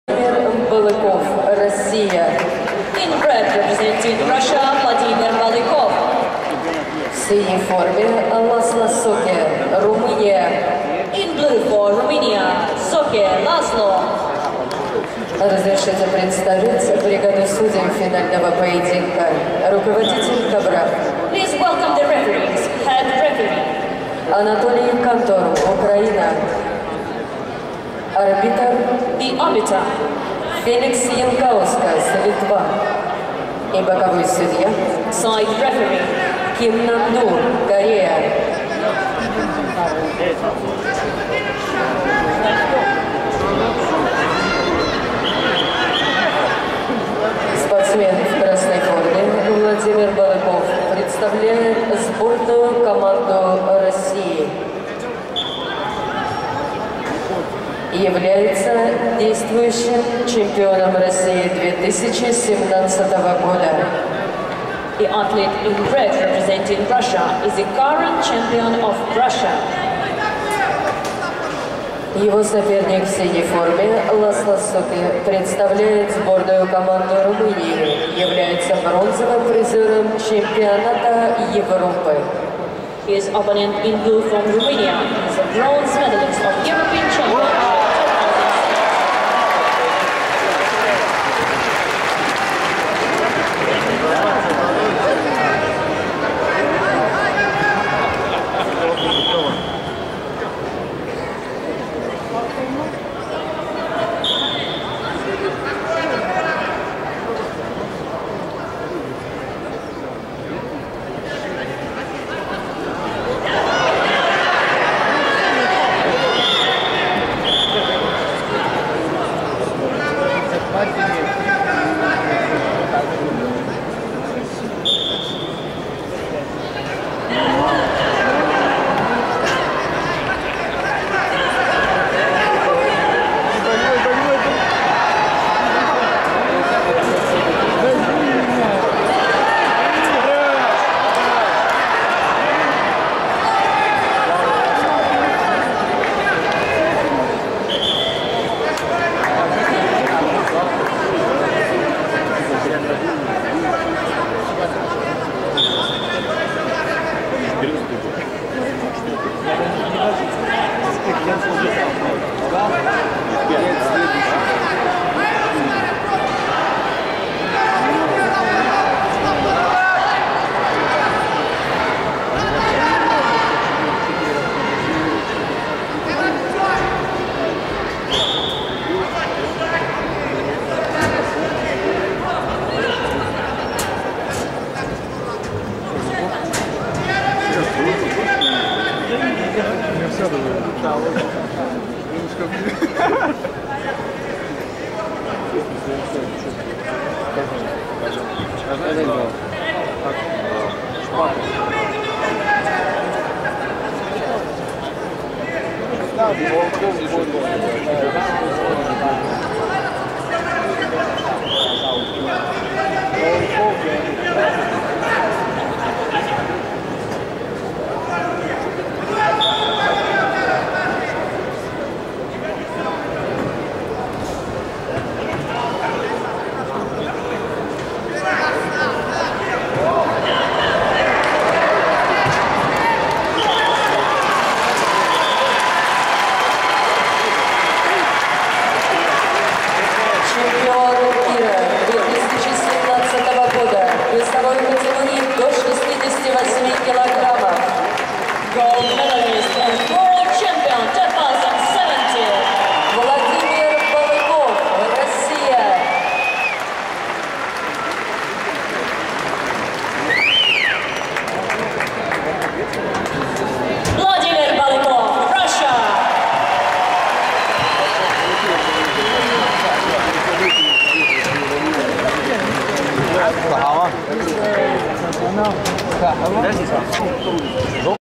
М68
Чемпионат мира по самбо 2017 года прошли в Сочи 9-13 ноября во дворце спорта «Айсберг». В соревнованиях приняли участие более 500 спортсменов из 90 стран мира.